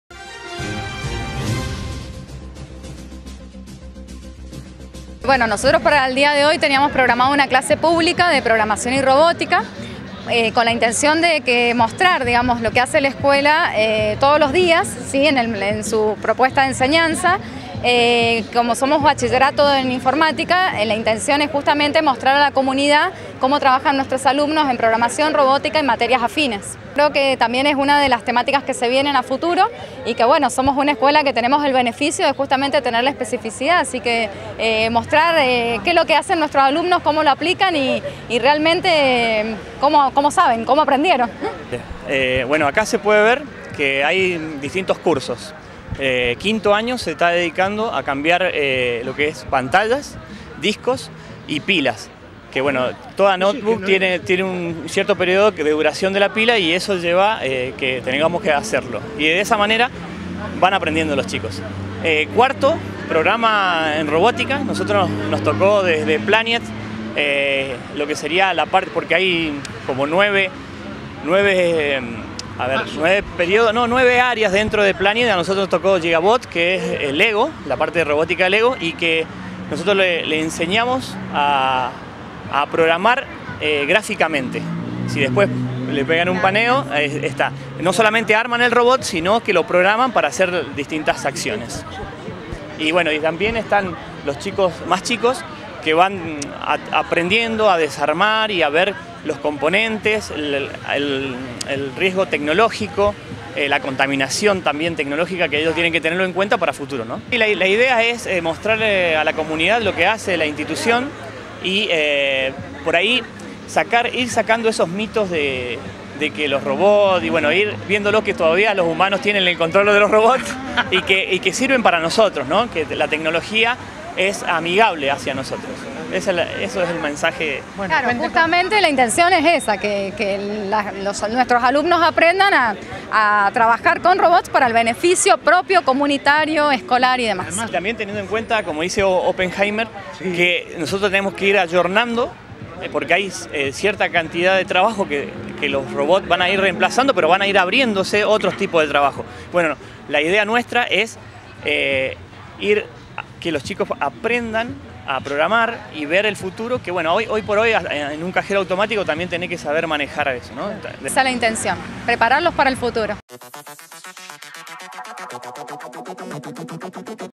MIREN LO QUE APRENDIMOS
La escuela "Jorge de la Reta" mostró en una clase pública lo que están haciendo los chicos en base a la robotica, y en la colaboración con otras instituciones escolares por el cambio de pilas de netbooks.